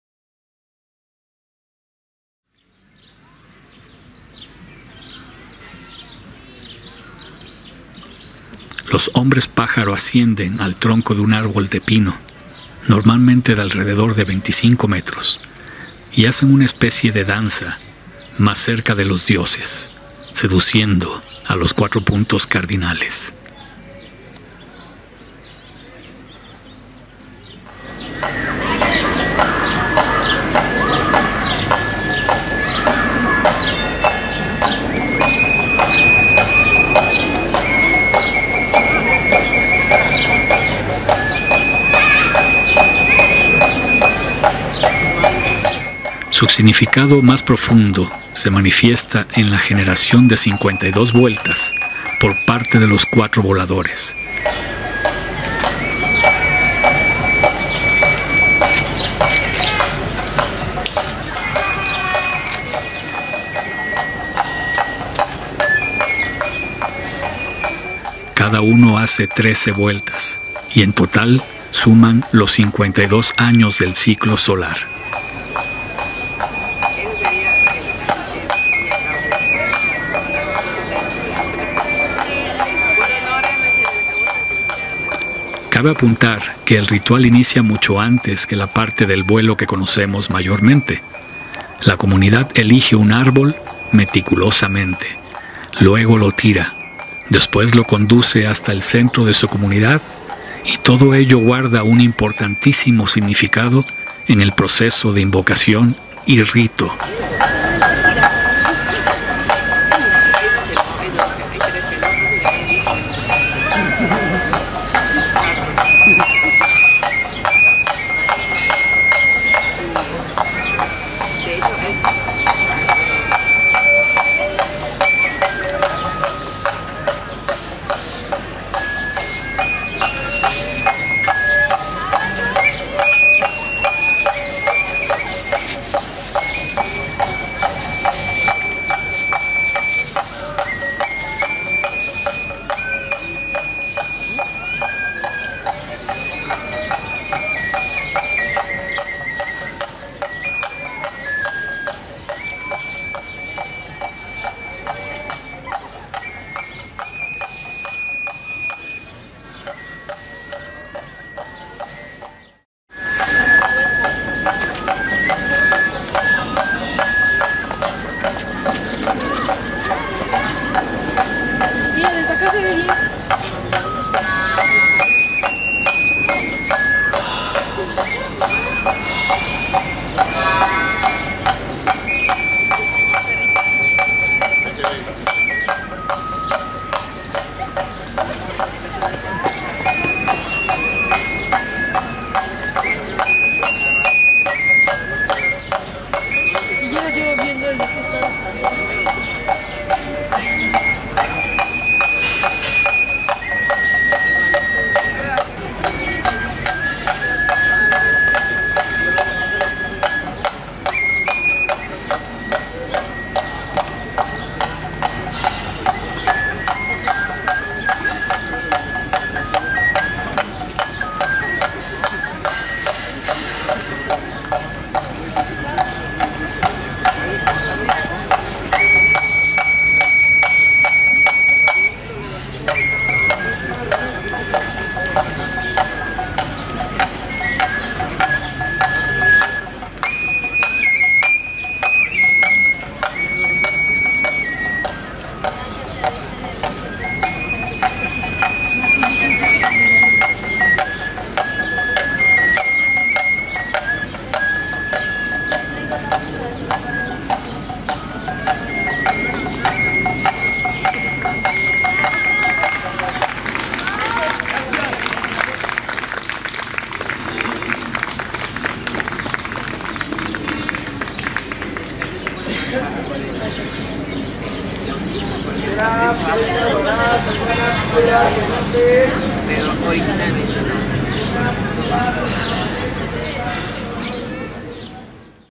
VOLADORES DE PAPANTLA
Gran historia ancestral de los Voladores de Papantla. Mira como van descendiendo, y hago una breve narrativa.